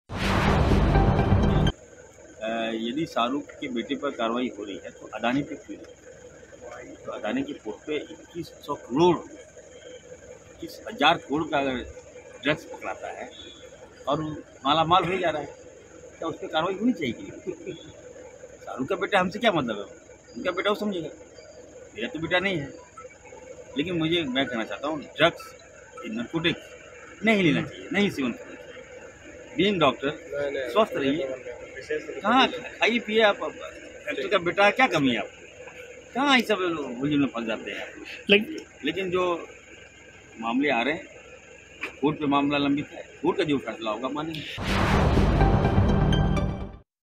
ड्रग्स मामले में इरफान अंसारी का बयान - आर्यन पर करवाई, तो अडानी पर क्यों नहीं ? | News Wing TV
Irfan khan rose questions on Central government.
He said why government is not taking any proceedings on Adani.
Gautam Adani has also been found with drugs, government should also take proceedings on him.